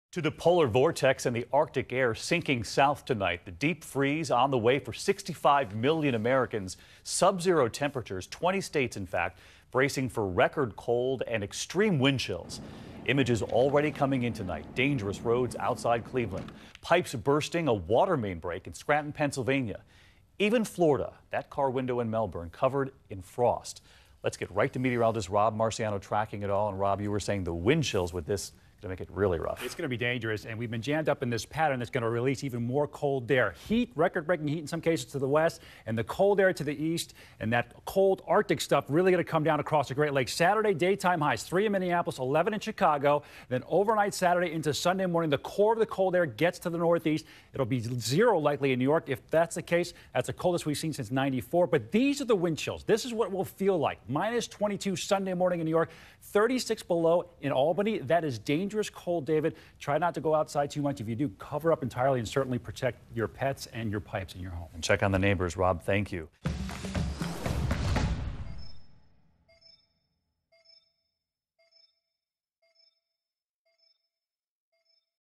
Deep Freeze For 65 Million Americans
20 states bracing for record cold, wind chills with dangerous road conditions.